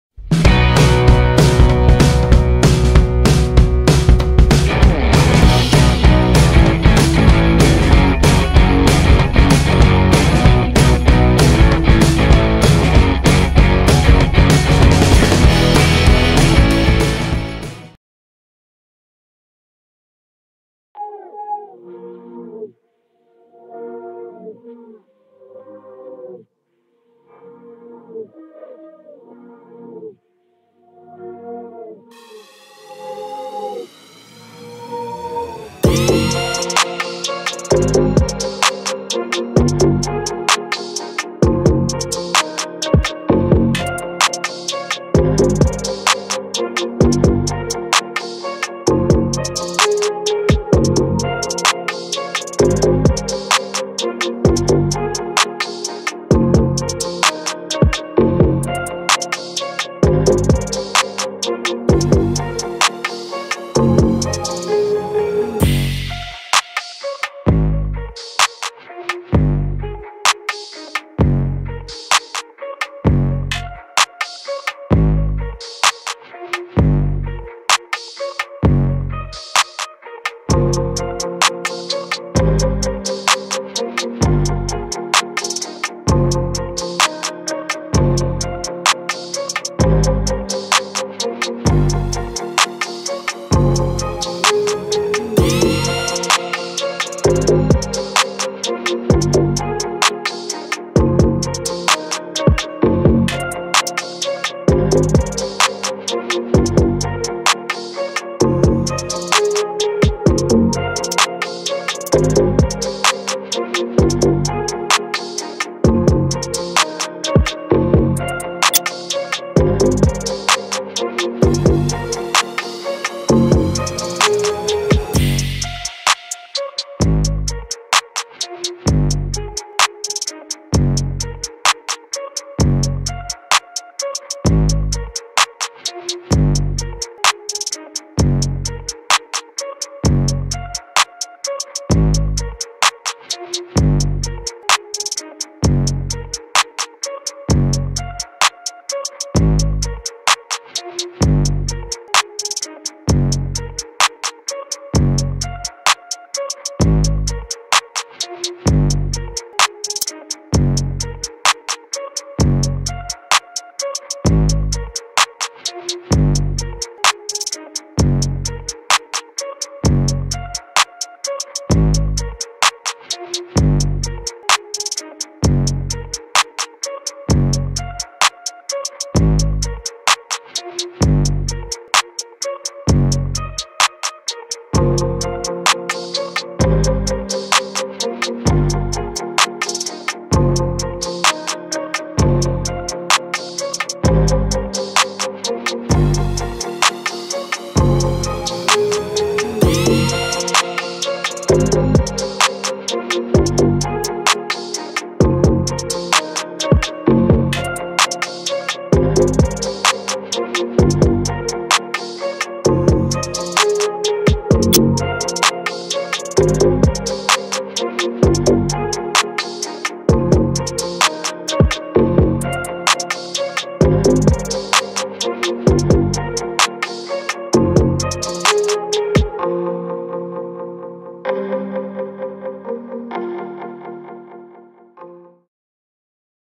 Kei Office Nissan Silvia S15 by Hotworks Racing
model: Nissan Silvia S15
variant: Kei Office
manufacturer: Hotworks Racing
series: D1 Grand Prix
scale: 1/64

360° view + photo slideshow